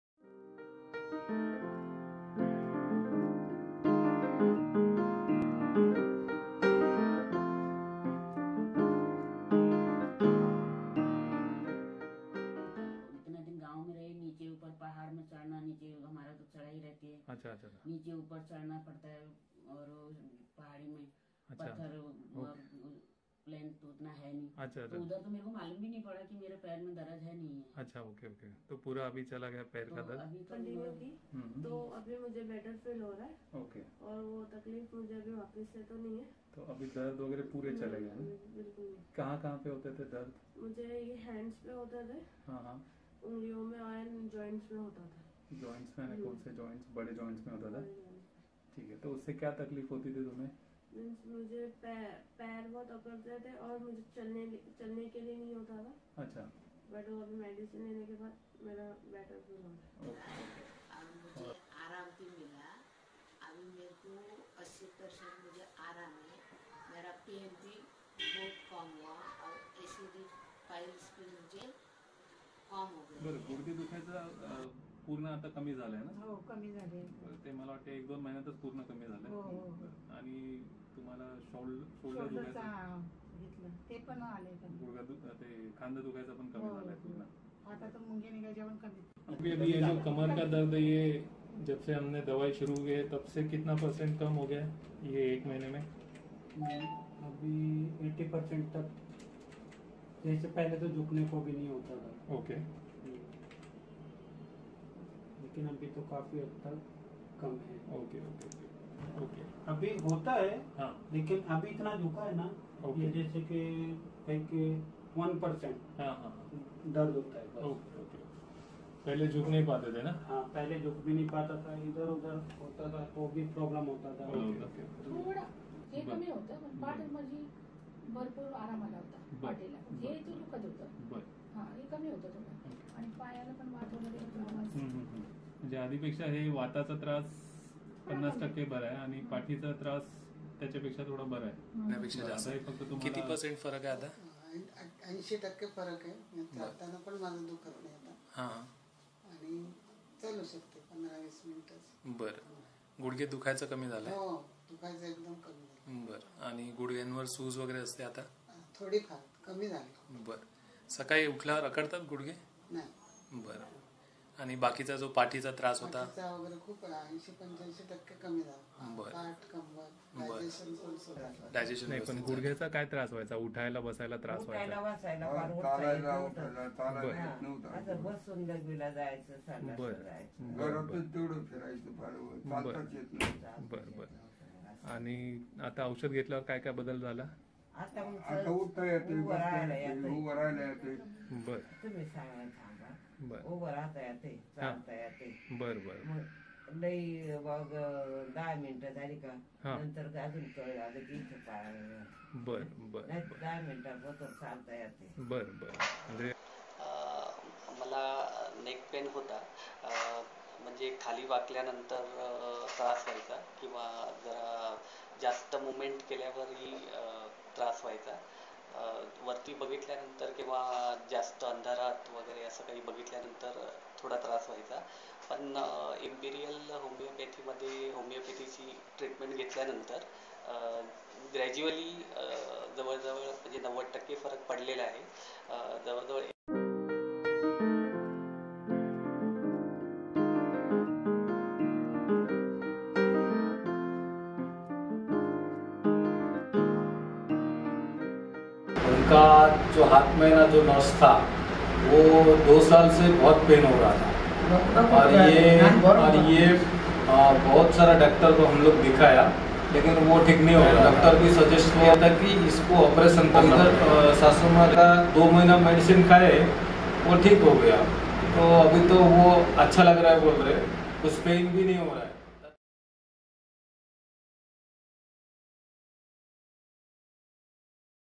Tired of pain killers? Start Homeopathy for your knee/neck/back/shoulder/heel/wrist joint pain.
Various patients from age group 19 to 76 years sharing their experience of relief in their joint pain after taking treatment from Dr. Pankaj Dere's Imperial Homeopathy Clinic, New Mumbai.
For appointment/order treatment online, call/whatsapp on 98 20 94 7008.
Dr. Pankaj Dere, M.D.